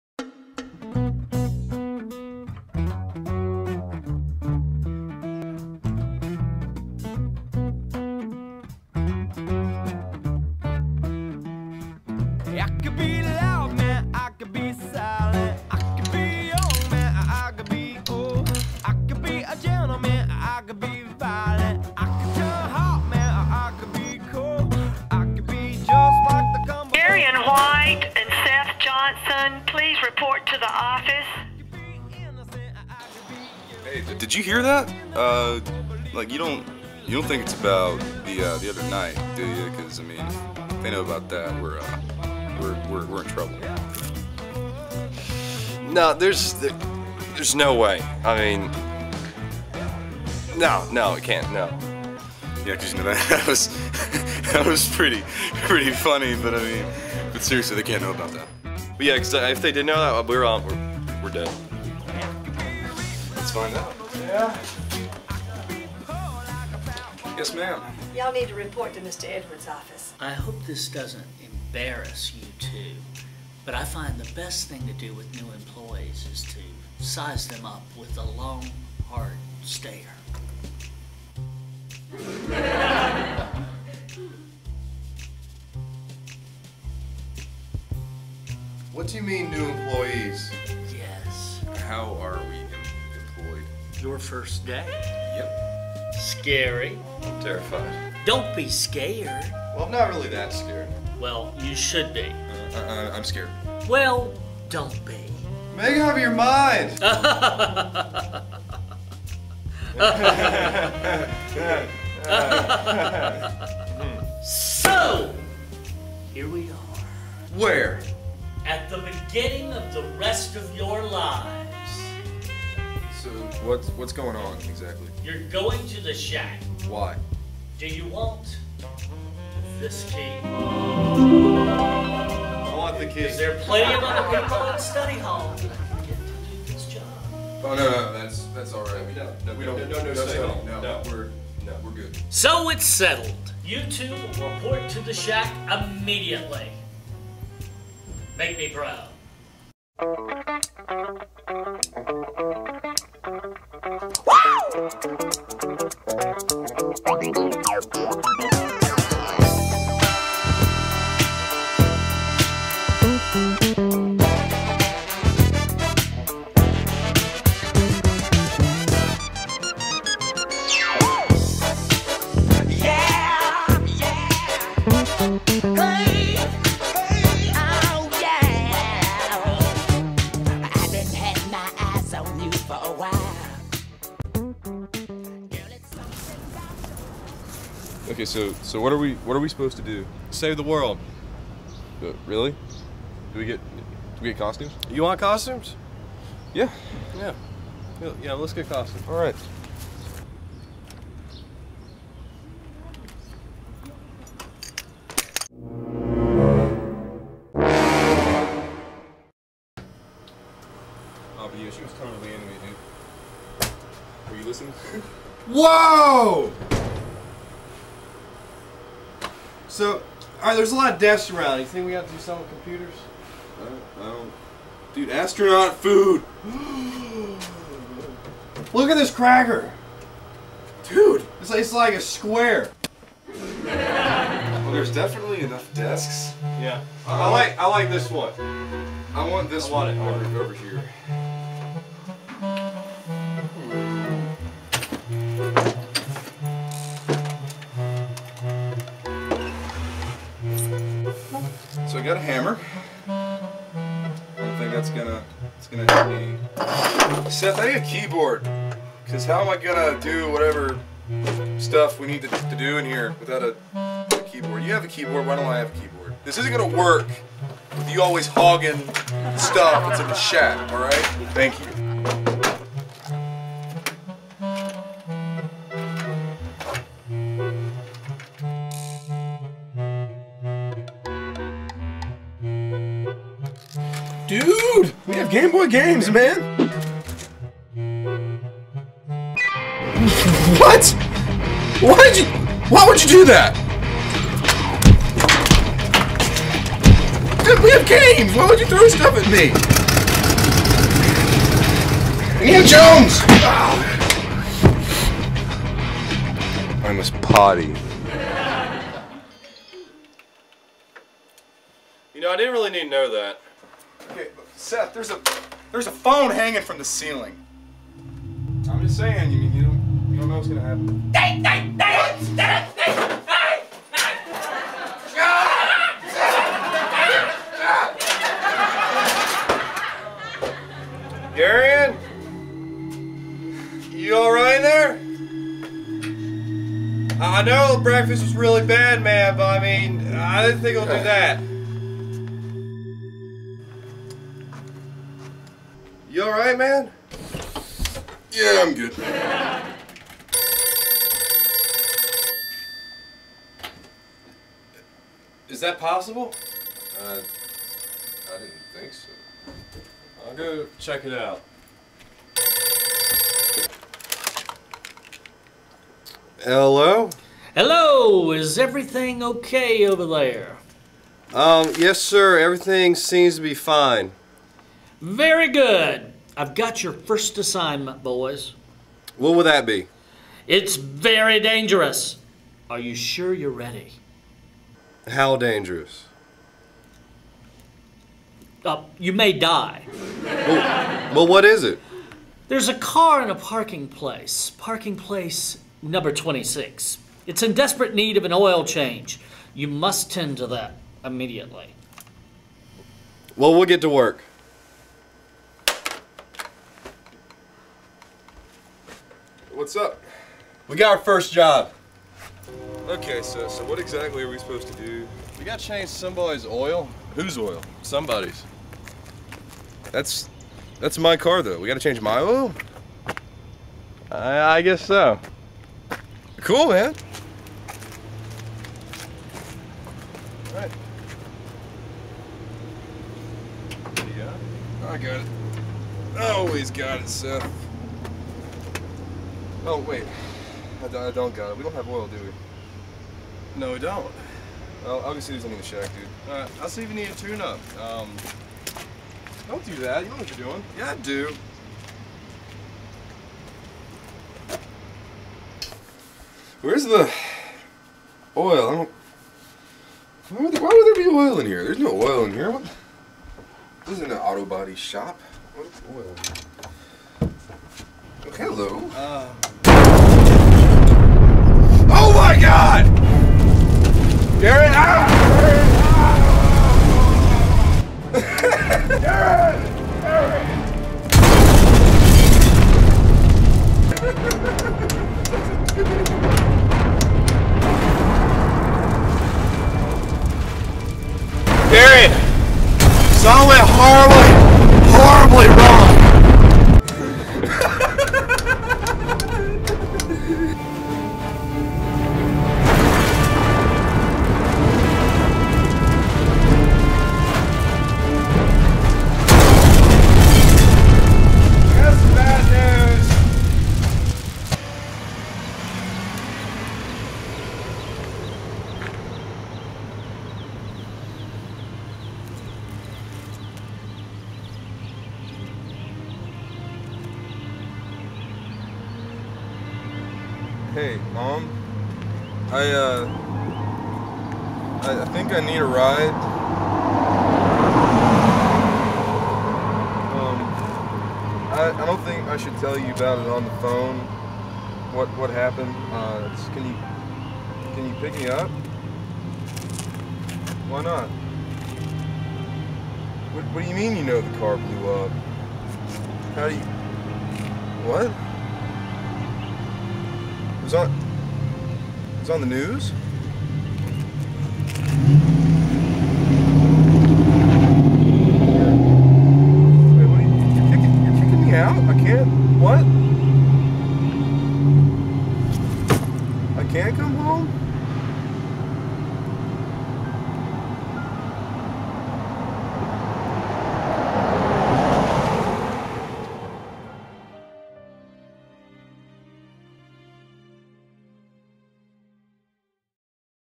Shack Jockies
A Short Film by Seth Johnson. Seth and Garion get called to the office and are sent to the shack just a few yards away from the Mayo Campus. They're not exactly sure why, or what they're suppose to do there. But they have no other choose but to go, or be sent to the dark depths of study hall. (Possible Series) -Pilot Episode-